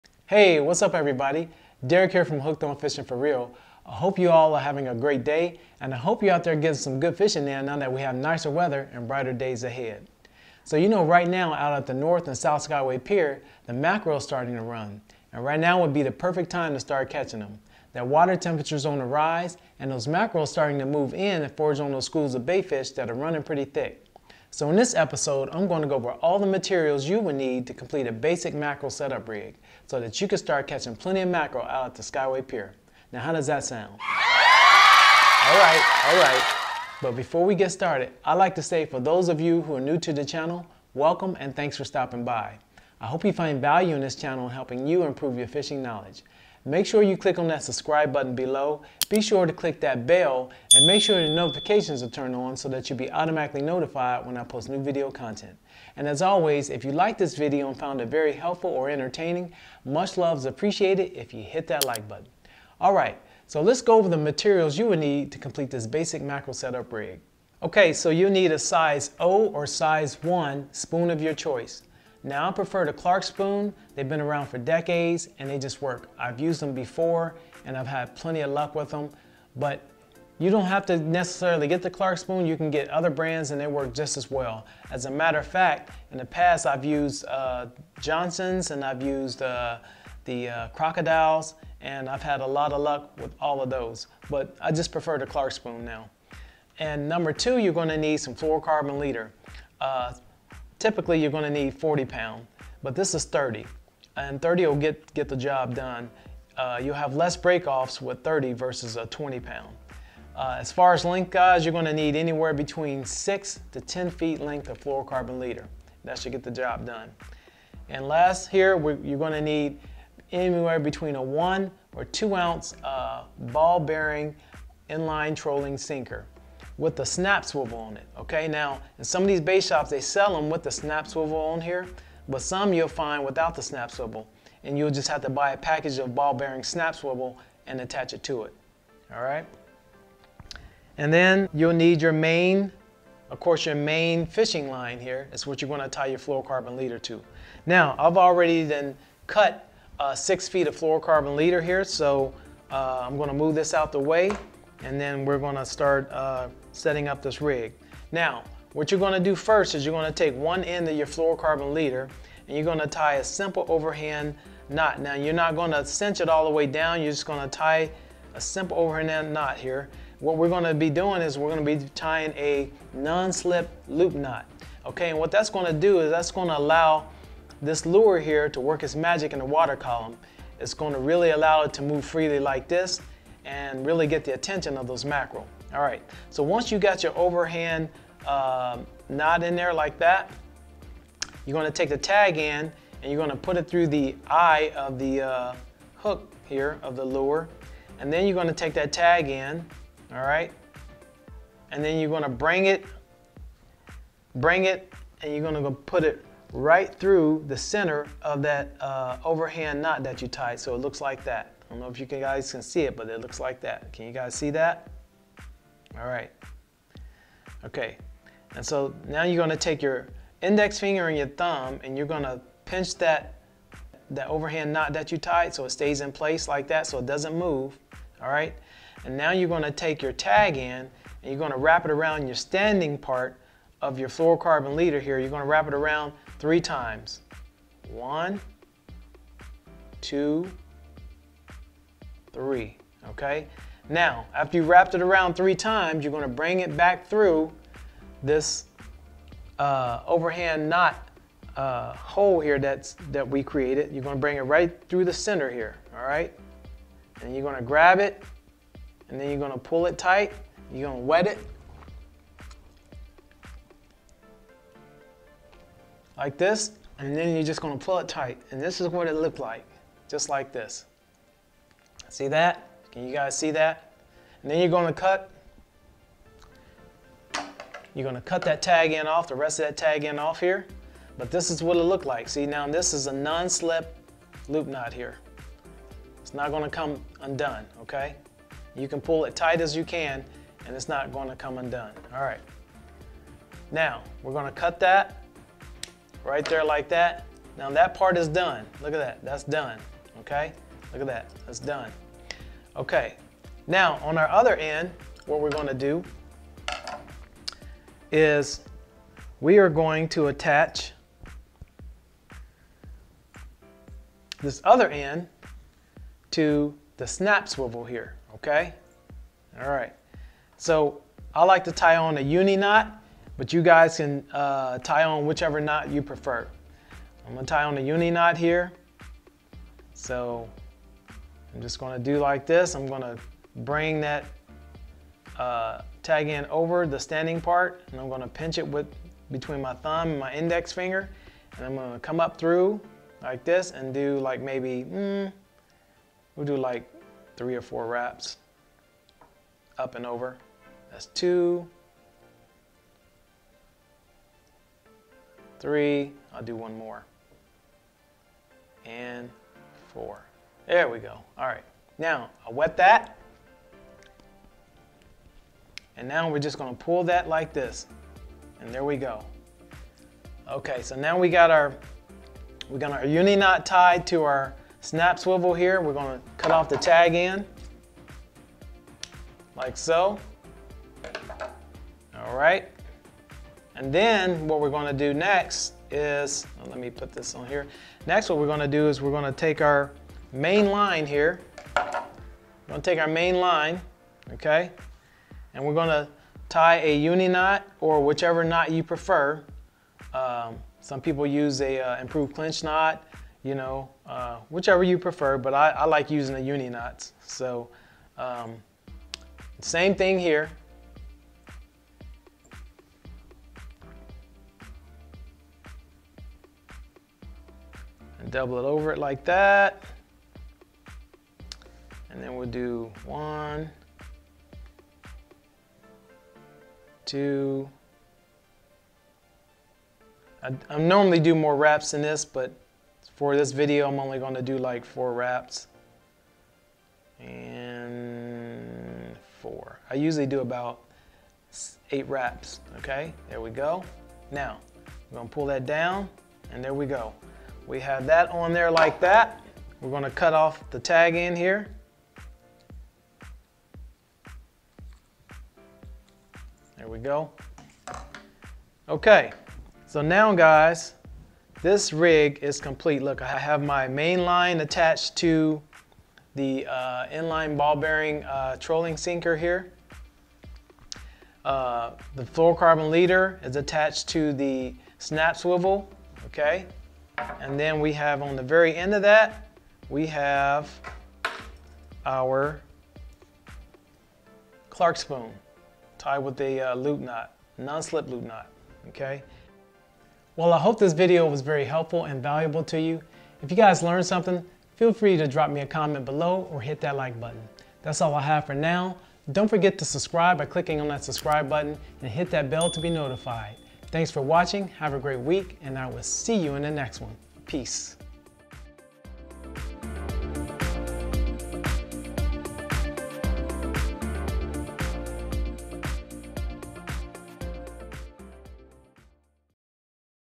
Pier Fishing: Learn How to Catch Spanish Mackerel at The Skyway Pier!
Learn How to Catch Spanish Mackerel at the Skyway pier using this simple Mackerel setup rig.